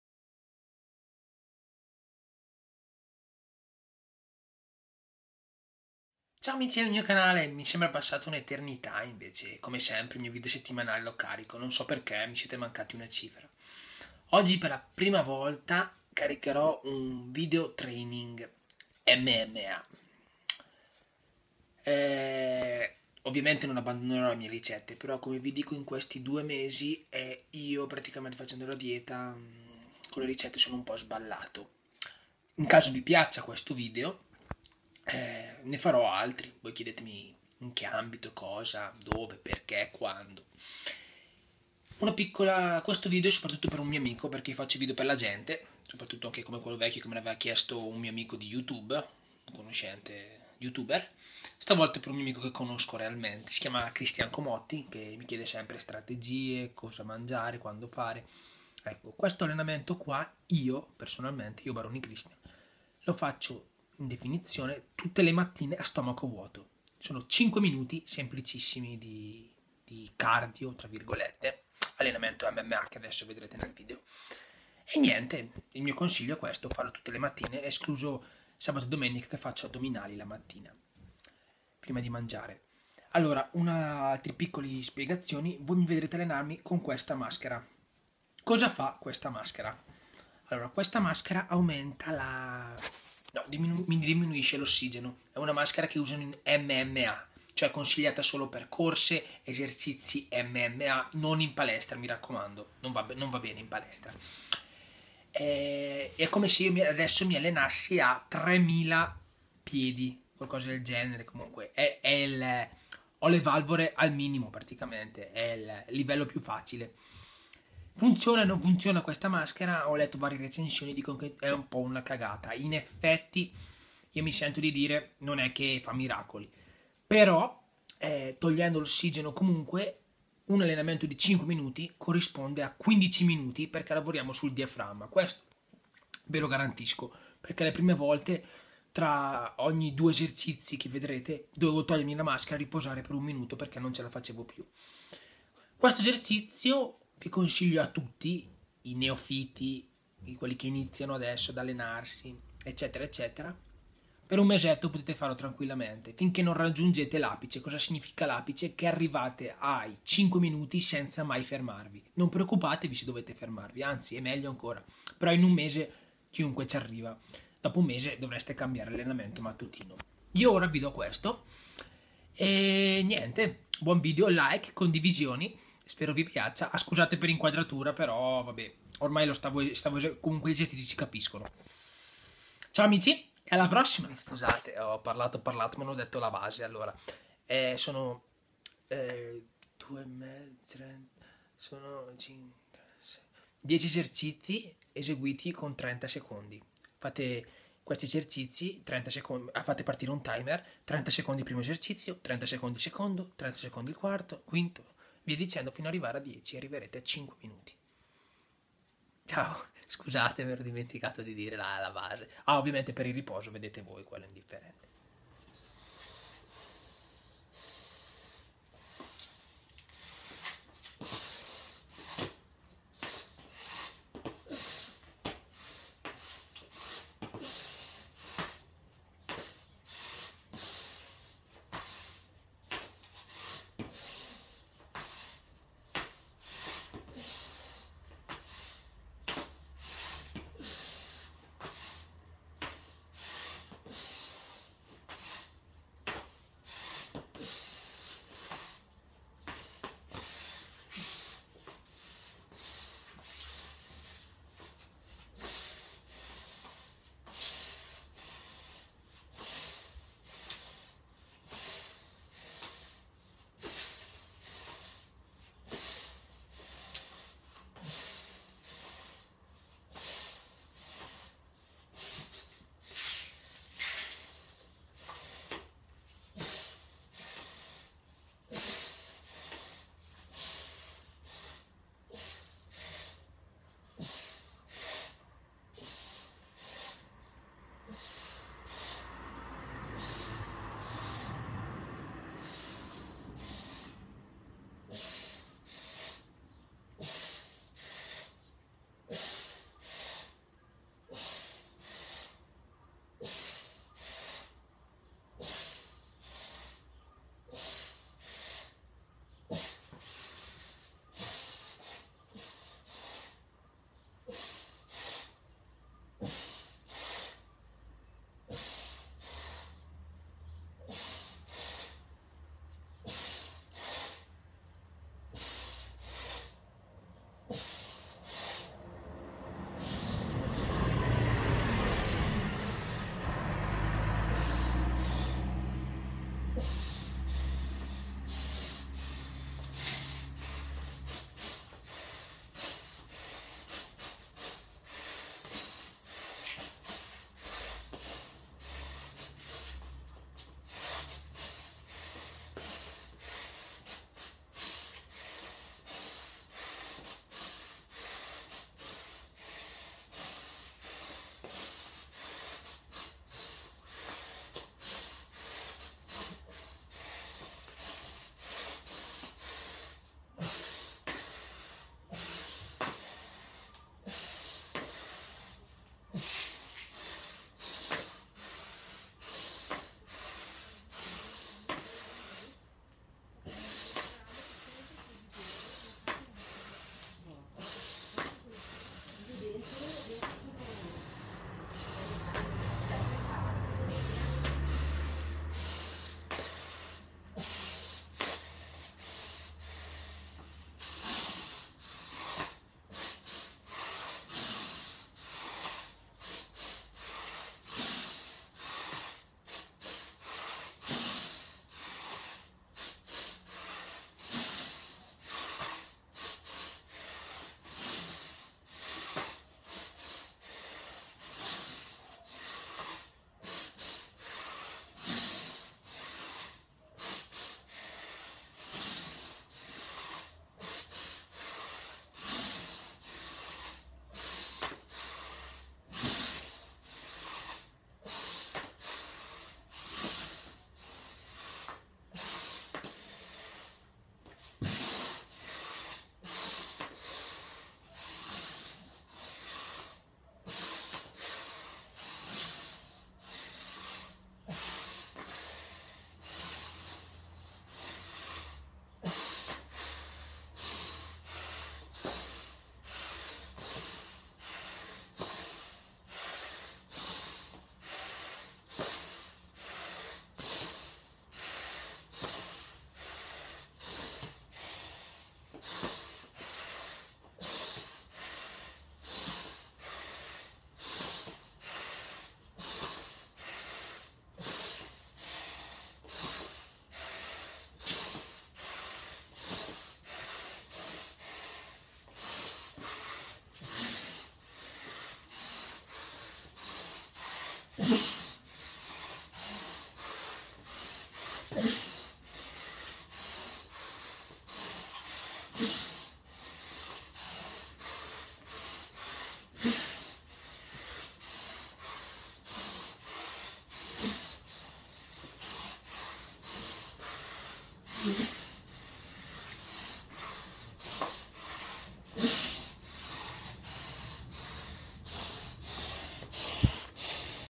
Training- Mma workout
Spero il video sia di vostro gradimento mi raccomando iscrivetetevi commentate e spolliciate like!!! Grasias.
Account Twitter: christian_ypro
Eseguire ogni esercizio per 30 secondi, possibilmente senza pause:
1 burpees
2 burpees thrusters
3 flessioni
4 v-ups
5 boxe
6 jump squat
7 sprawls
8 burpees
9 affondi alternati
10 burpees laterali